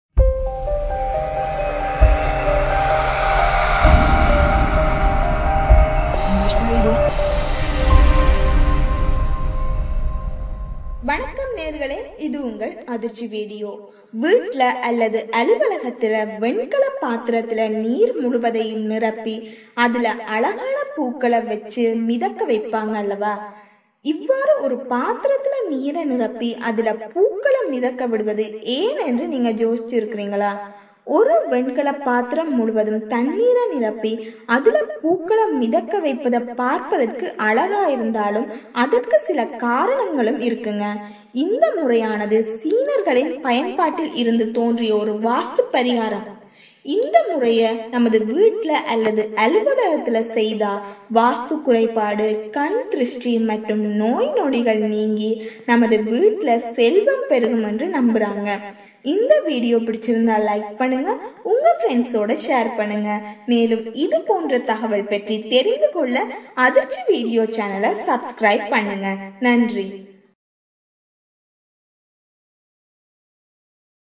வீட்டில் பாத்திரத்தில் நீர் ஊற்றி பூக்களை மிதக்க விடுவது ஏன்? உங்களுக்கு தெரியுமா?
water to float the flowers, and why? Did you know?